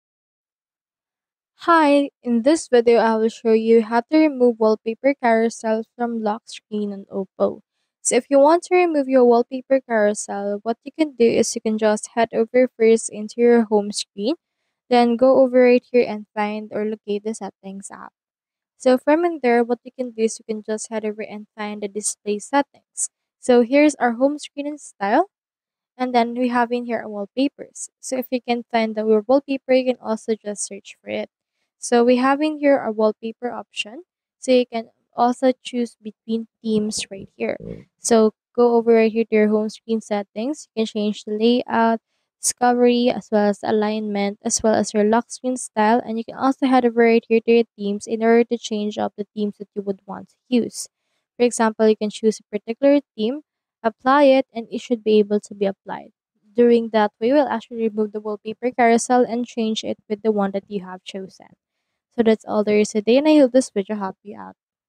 How to Remove Wallpaper Carousel From Lock Screen Oppo
📌 In this video, I'll show you how to remove Wallpaper Carousel from your phone’s lock screen in just a few simple steps. This method is quick, easy, and clearly explained, so you can follow along and get it done in no time!

🔹 Related Videos on my Channel